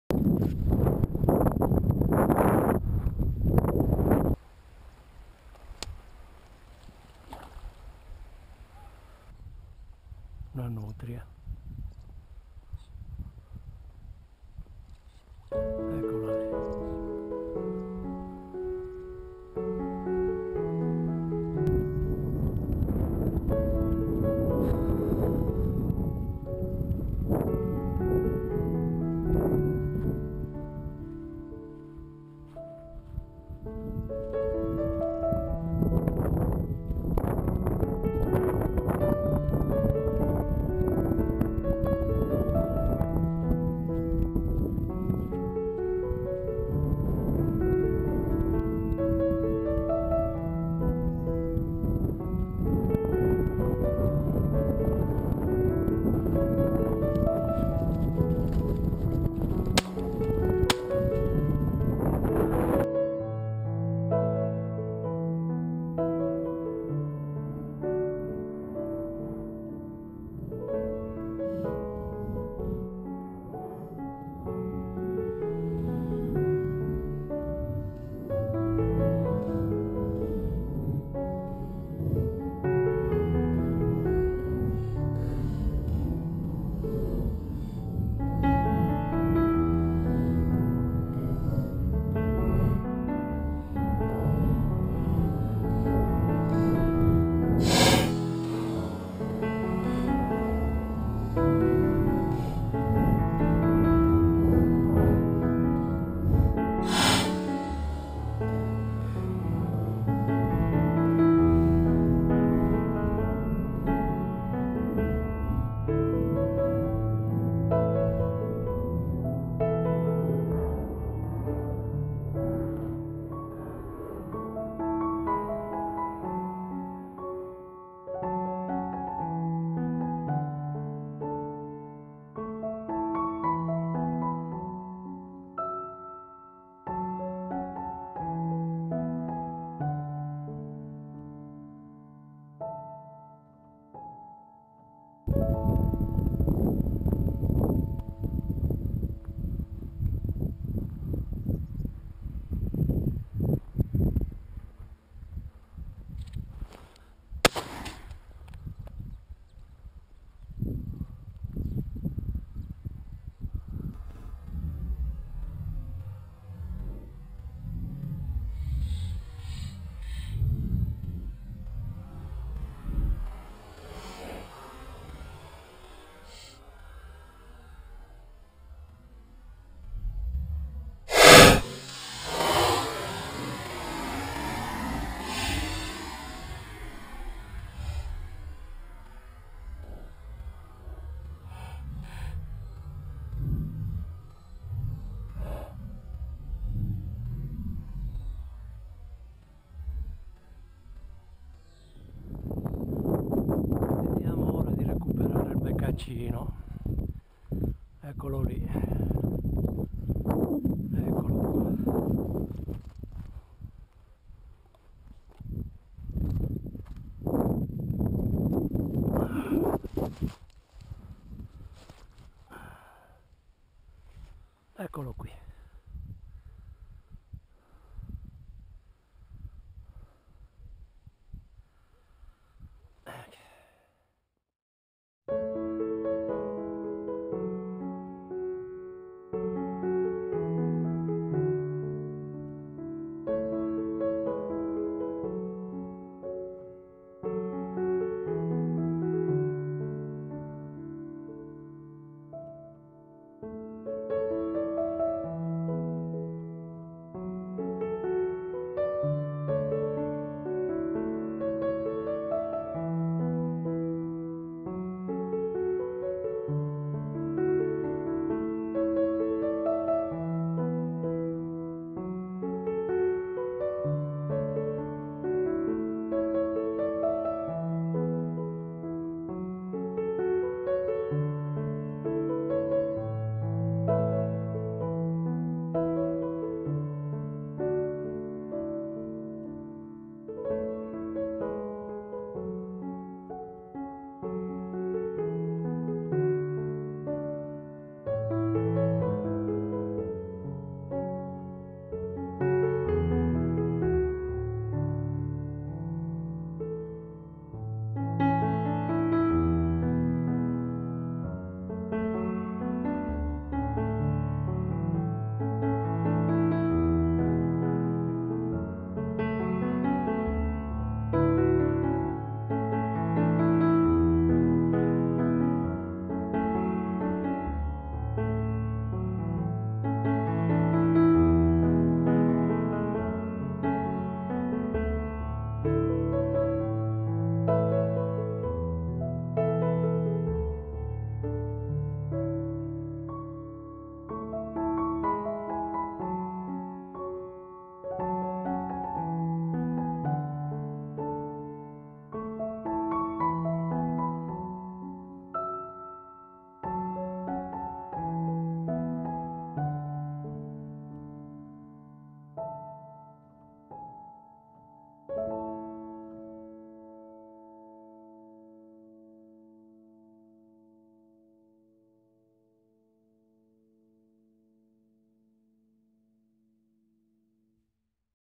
Giornate di Caccia 21/01/2021
Una delle ultime giornate di Caccia di una stagione molto travagliata, con poca selvaggina, brutto tempo e molte giornate perse causa lockDown.